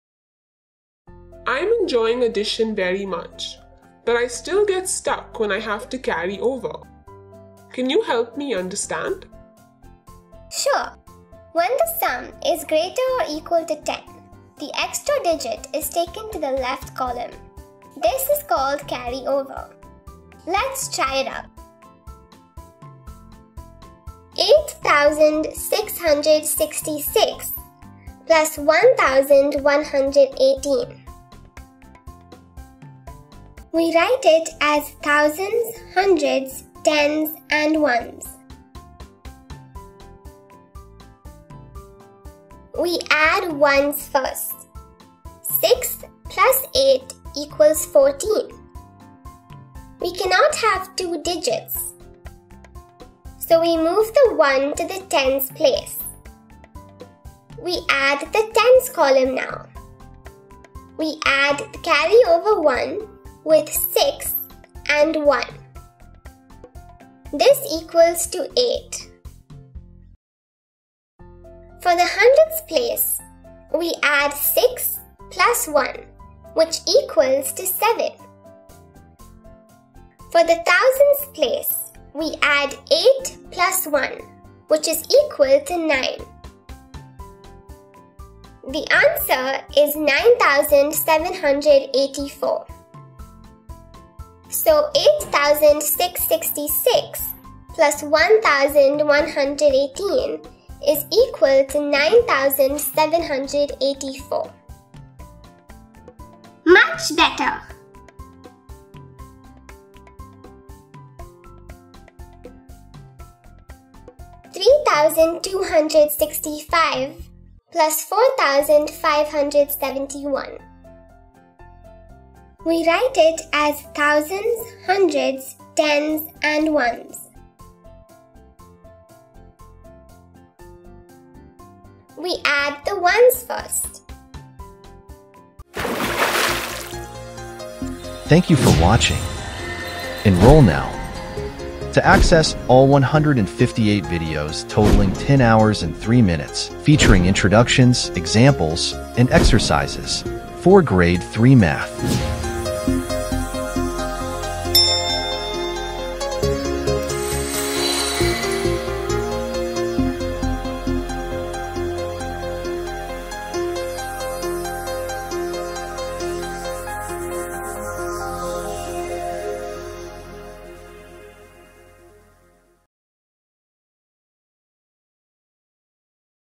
G3 - Module 2 - Exercise 2 - Addition with Carry Over | Appu Series | Grade 3 Math Learning
It's practice time! Level up your addition with carry over skills in this fun exercise with Appu and Tiger. Follow the steps with confidence and add larger numbers with ease! The more you work out, the better you'll get!